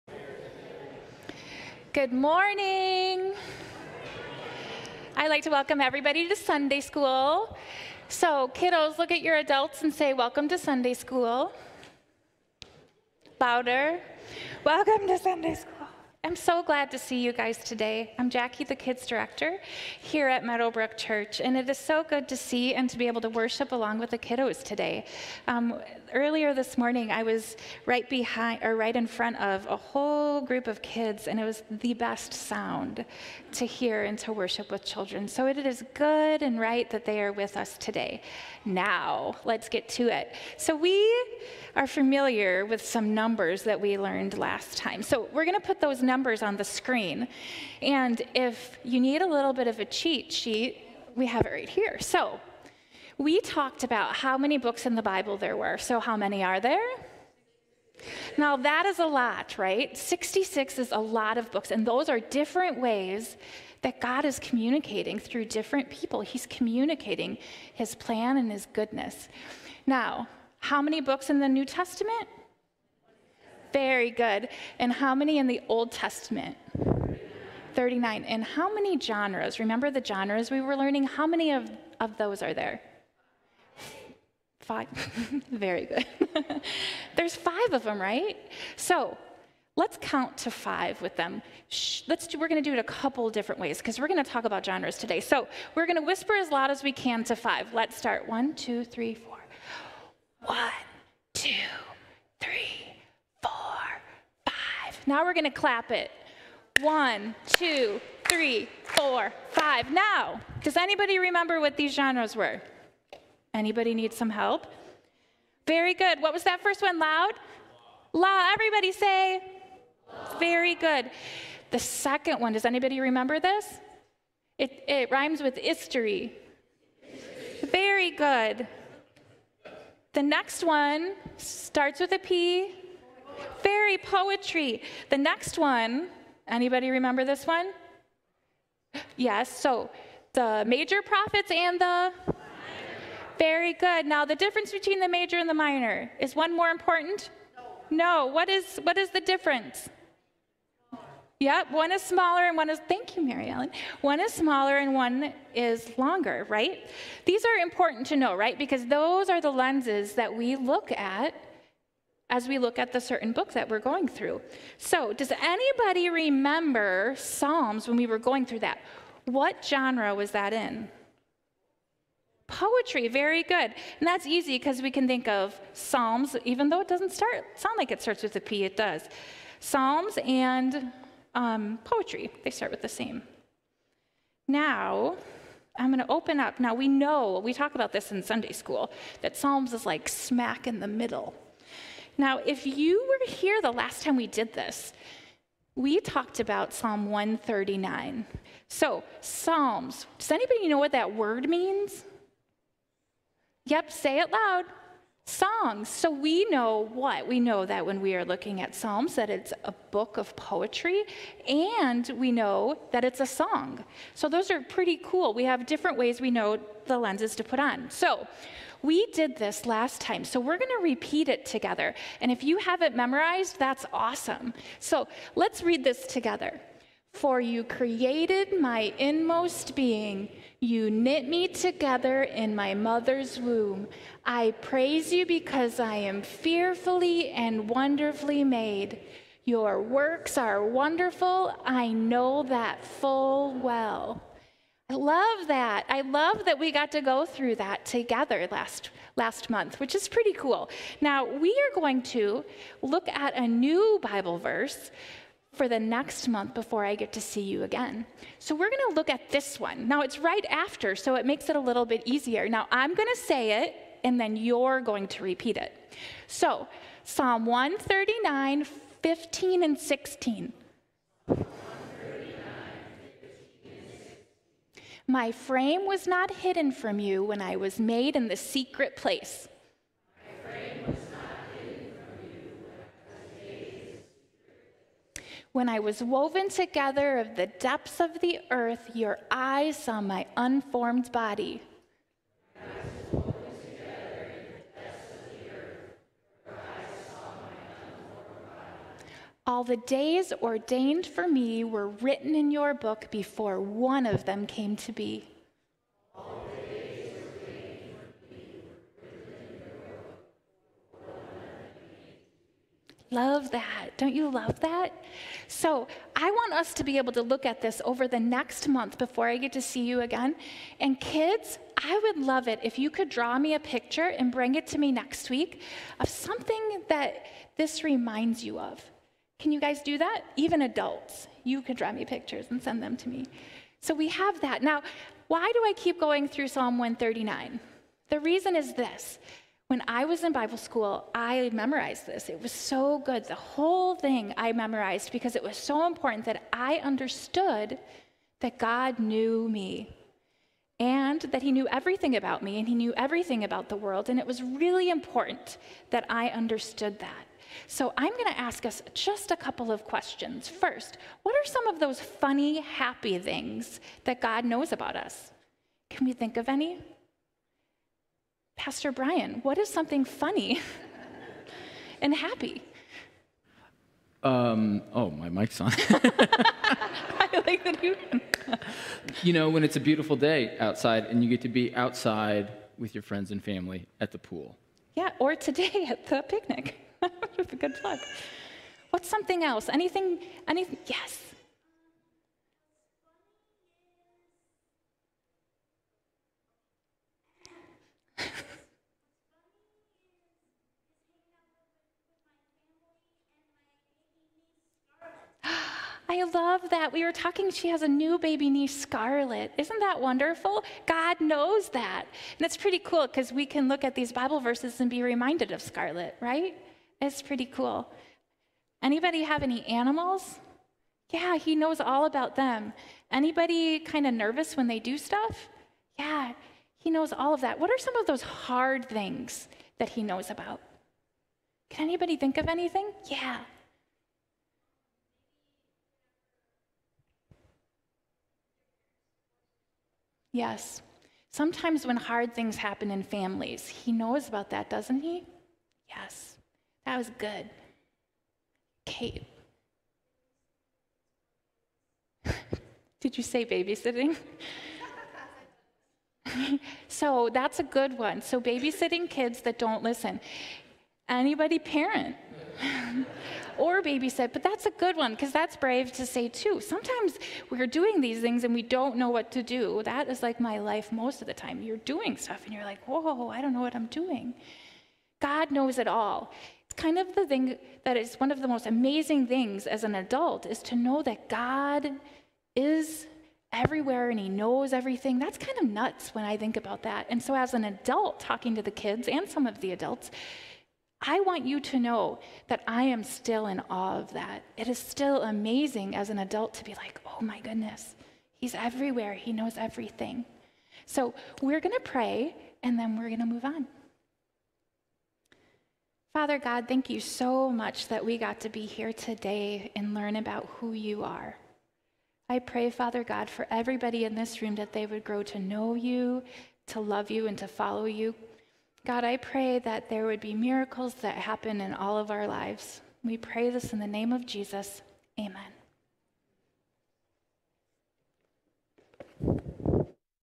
Chosen, Created, and Loved
Children's Director Ms. Jackie shares the promises of Psalm 139

Follow MBC!!